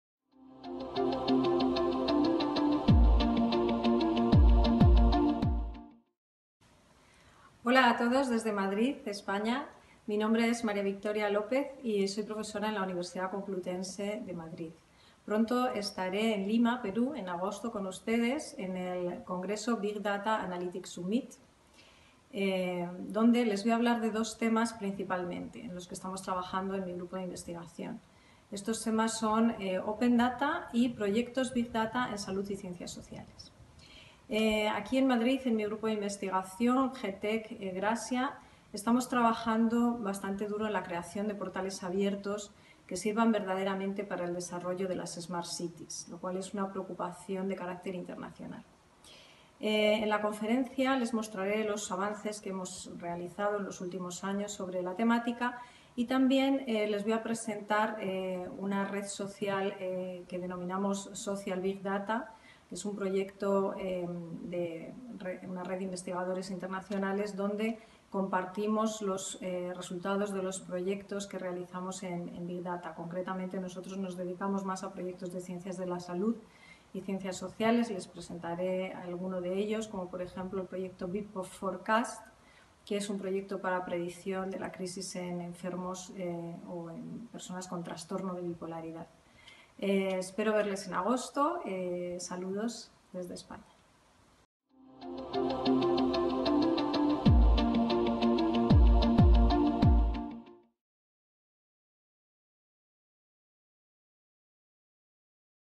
Big Data Analytics Summit - Victoria Lopez
Victoria Lopez, Docente de la Universidad Complutense nos compartirá su experiencia en el tema de Big Data. Además, nos hablará de sus proyectos para las smart cities.